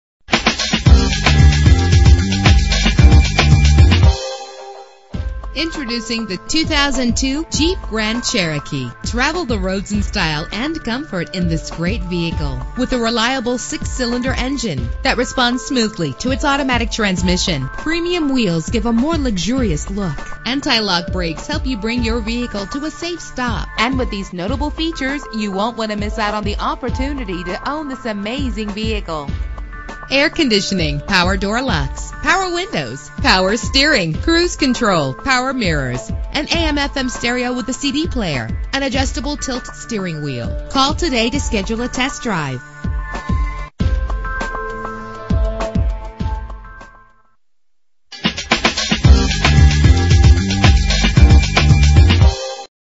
2002 Jeep Grand Cherokee - Jefferson City MO
Year: 2002
Make: Jeep
Model: Grand Cherokee
Trim: Laredo
Engine: 4.0 liter inline 6 cylinder 12 valve
Transmission: Automatic
Color: Gray
Mileage: 140336
Address:
2033 Christy Dr